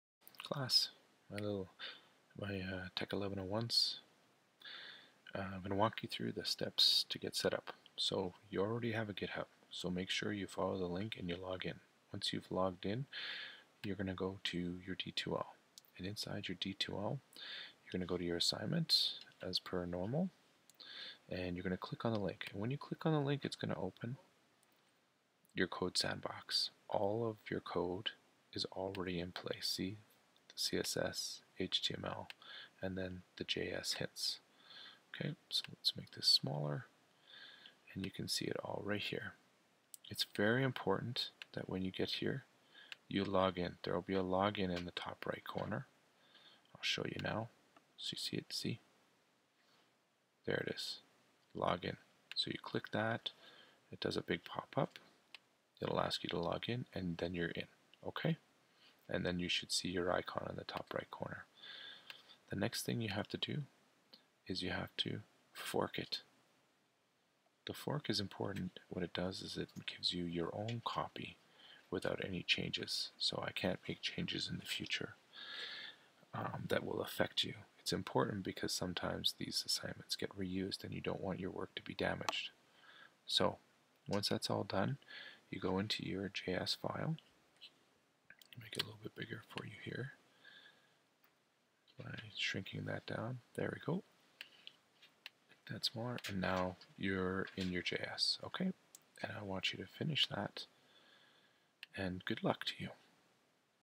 How to submit code when we are using CodeSandbox!
Are you trying to determine how to submit an assignment that was developed on CodeSandbox? This is how you submit.

Don't forget to FORK it!